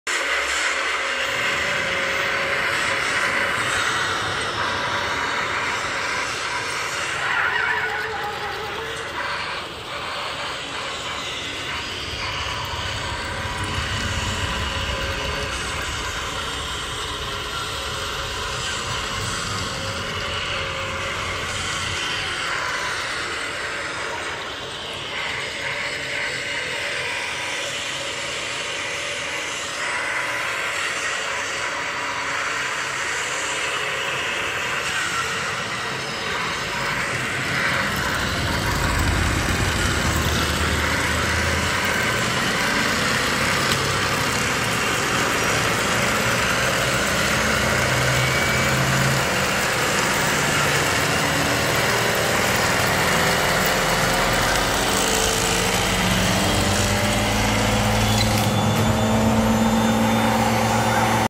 F1 Trucks fighting for the victory
Formula 1 TRUCKS

Direct battle between trucks loaded with Formula 1 cars (Lotus and Williams).
Caminhões F1

Batalha direto entre caminhões carregados com carros de Fórmula 1 (Lotus e Williams).